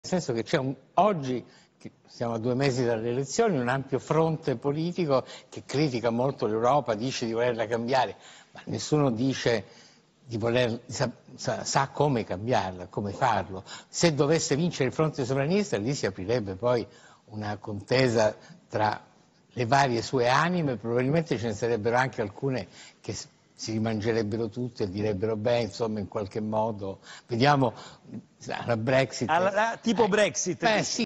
Galli Della Loggia: ‘Tutti vogliono cambiare l’Europa ma nessuno sa come farlo’
Ernesto Galli Della Loggia: ‘Se dovessero vincere i sovranisti, qualcuno di loro potrebbe rimangiarsi tutto per evitare l’effetto Brexit’